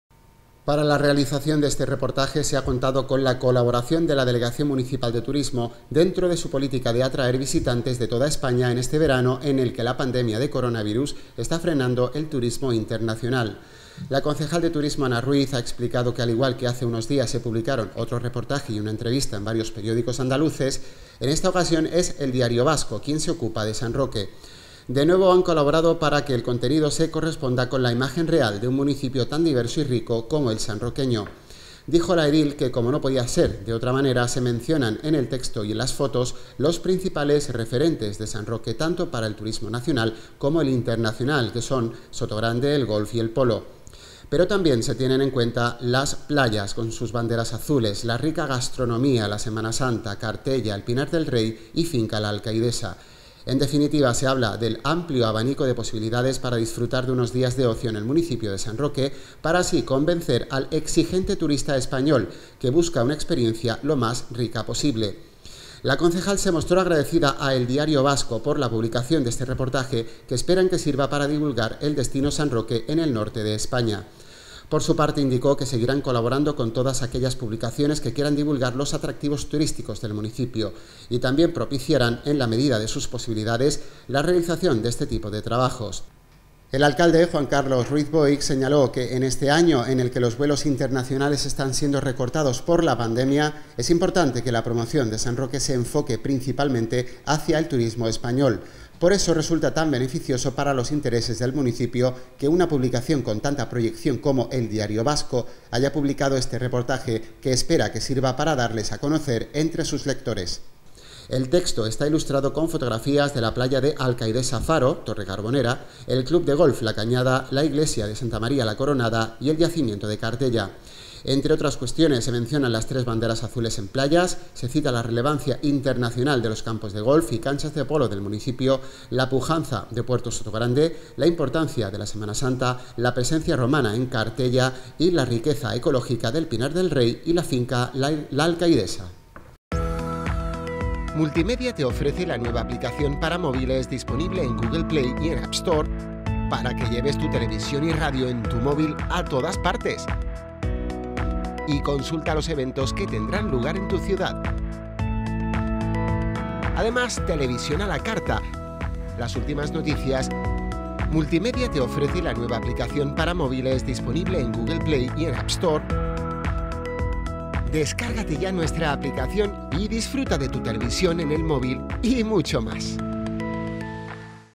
NOTICIA REPORTAJE TURISMO DIARIO VASCO 3 JULIO 2020
“El Diario Vasco” publica un reportaje turístico en el que se destaca la riqueza y variedad de San Roque
“El Diario Vasco”, uno de los principales periódicos del norte de España, ha publicado un reportaje titulado “San Roque, una joya del turismo andaluz”, en el que se recorren los principales atractivos del municipio. Para la realización de este reportaje se ha contado con la colaboración de la Delegación Municipal de Turismo, dentro de su política de atraer visitantes de toda España en este verano en el que la pandemia de coronavirus está frenando el turismo internacional.